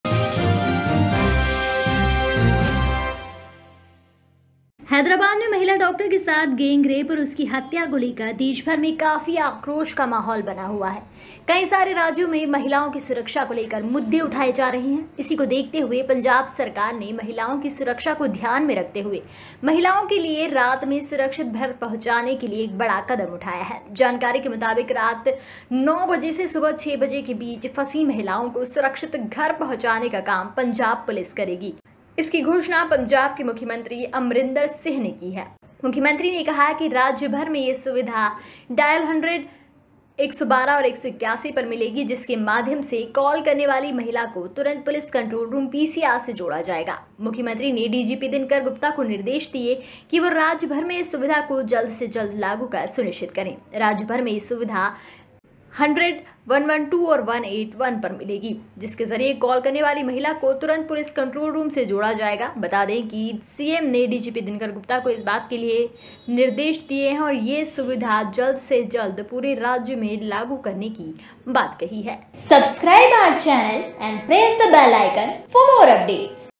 रात में Punjab में महिलाओं को पुलिस सुरक्षित छोड़ेगी | Punjab CM Captain Amrinder Singh | Ghamasan
पंजाब के मुख्यमंत्री ने किया यह ऐलान |